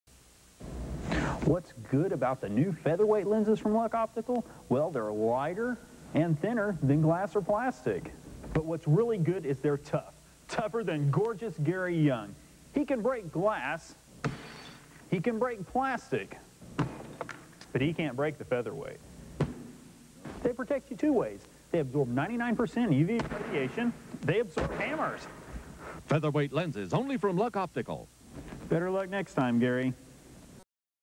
Gary Young commercial
Gary Young doing a local commercial in texas for some company that does lenses from 1990.